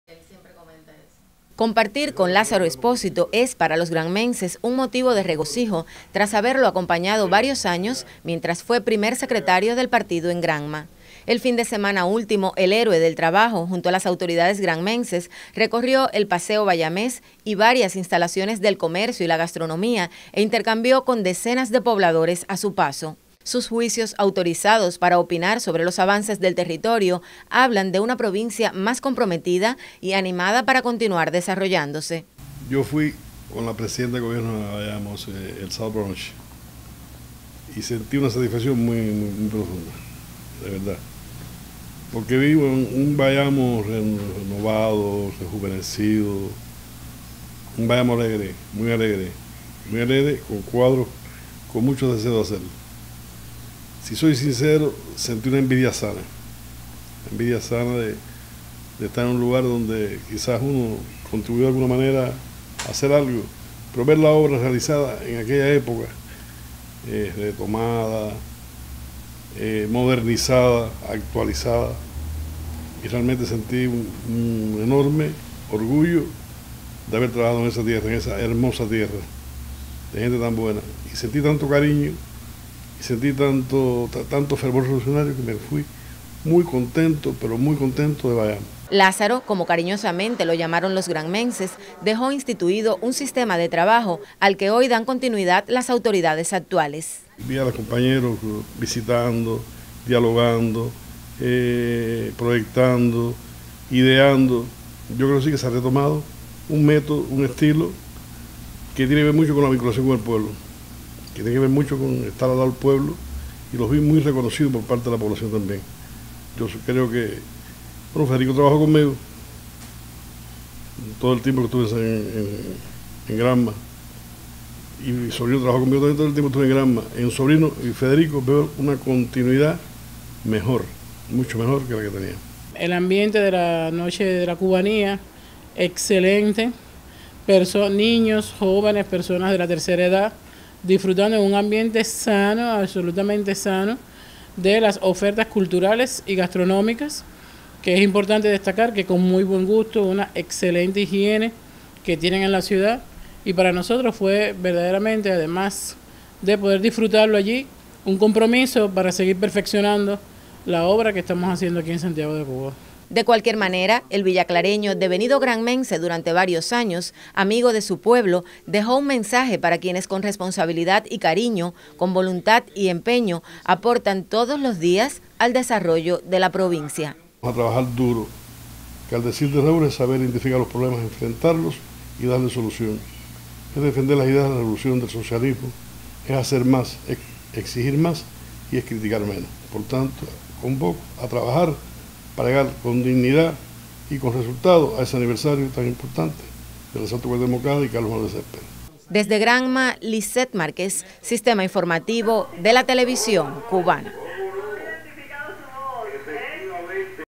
Entrevista con lázaro Expósito Canto
Una placentera visita recibimos los granmenses el fin de semana último.  El Héroe del Trabajo de la República de Cuba, y Primer Secretario del Partido en la hermana provincia de Santiago de Cuba, Lázaro Expósito Canto, quien fuera durante varios primer secretario del Partido aquí en Granma, recorrió el paseo bayamés, y otras instalaciones de la ciudad capital granmense, junto a Beatriz Jhonson Urrutia, Presidenta del Gobierno en ese territorio oriental.  La periodista Lizet Márquez conoció de sus impresiones y consideraciones, tras su visita.